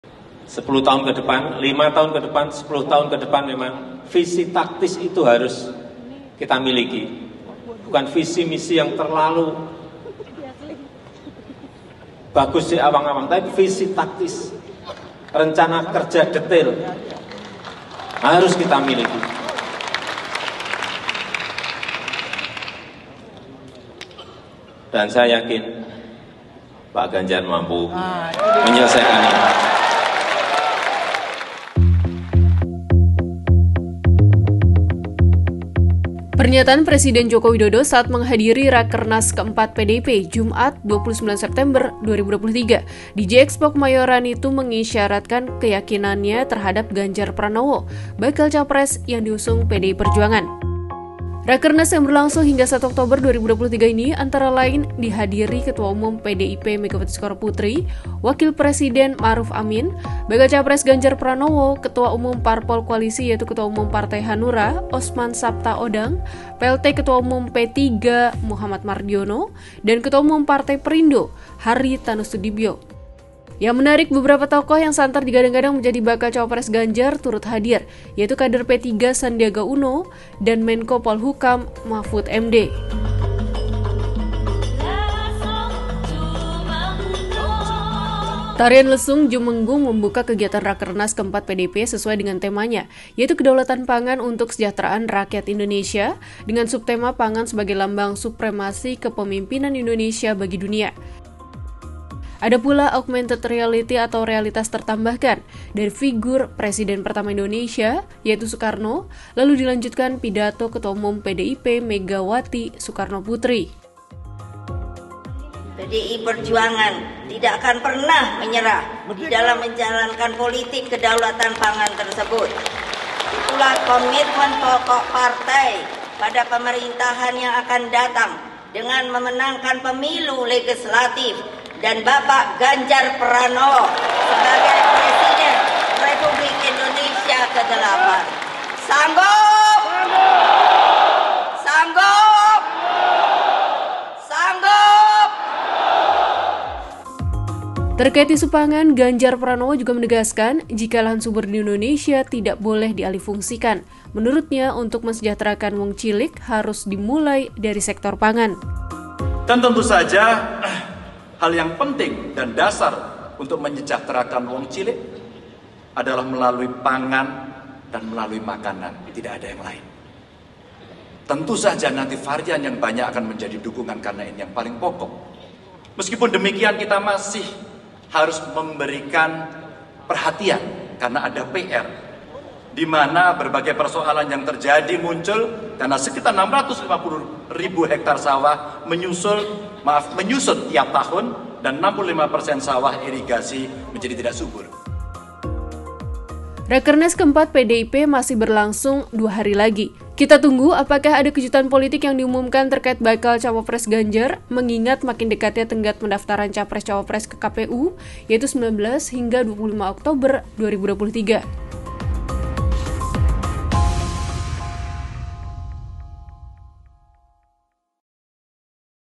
Hadiri Rakernas PDI-P, Jokowi Sebut Ganjar Mampu Laksanakan Visi Taktis
Pernyataan Presiden Joko Widodo saat menghadiri Rakernas ke-4 PDI-P, Jumat (29/9/2023), di Jiexpo Kemayoran, itu mengisyaratkan keyakinannya terhadap Ganjar Pranowo, bakal capres yang diusung PDI Perjuangan.